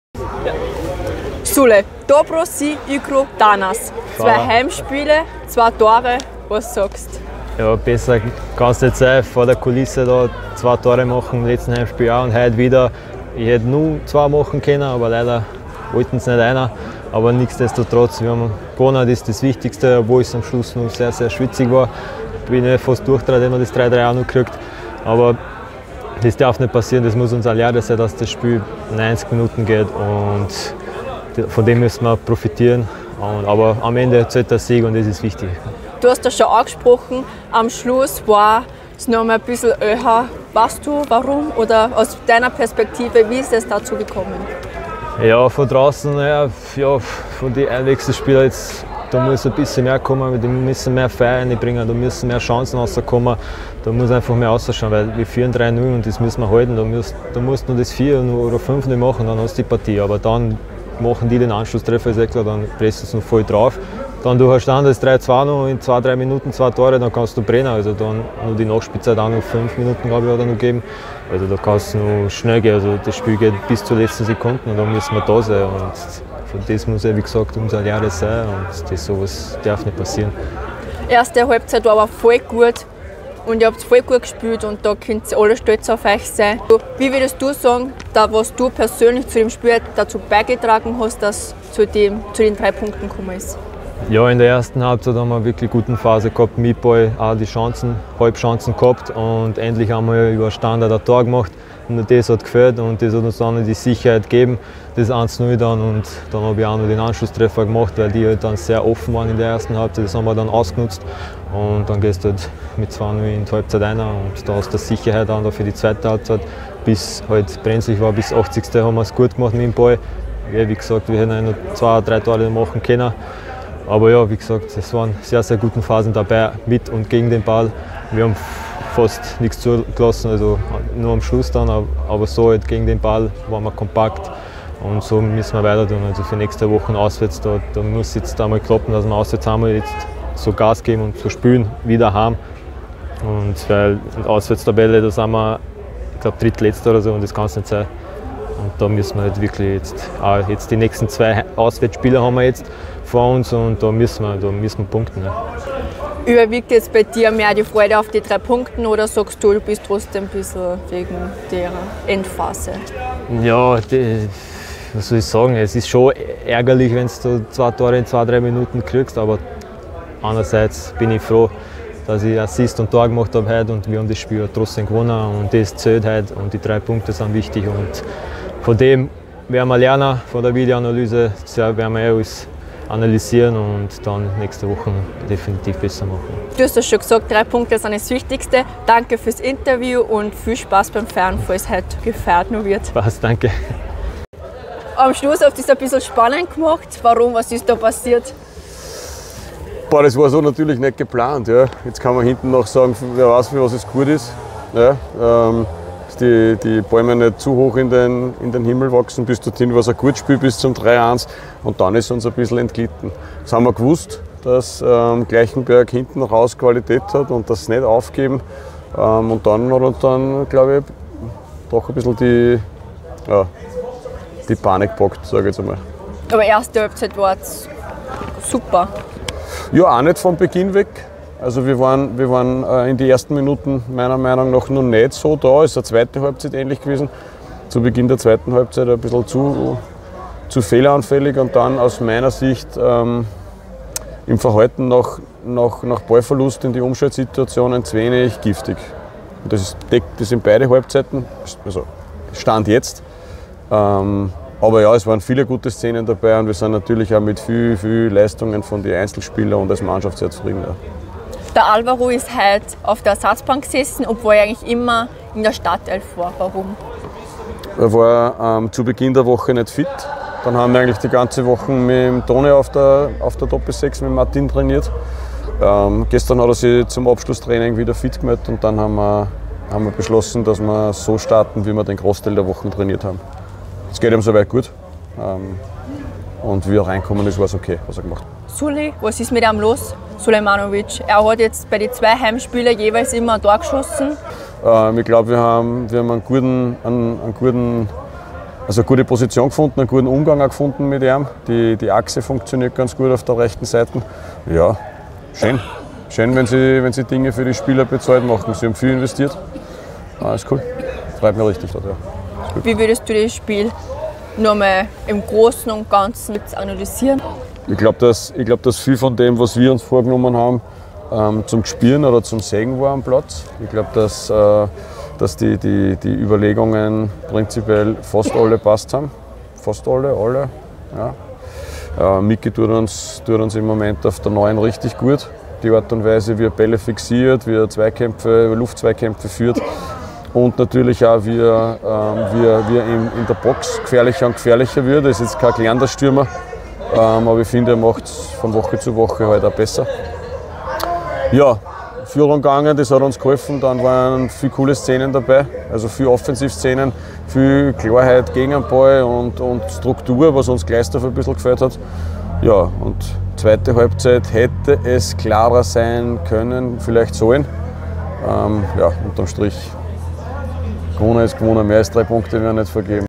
Stimme zum Heimsieg | Mirsad Sulejmanovic und Markus Eitl
Torschütze Mirsad Sulejmanovic und Trainer Markus Eitl zum 3:2 des SK BMD Vorwärts Steyr gegen TuS Bad Gleichenberg.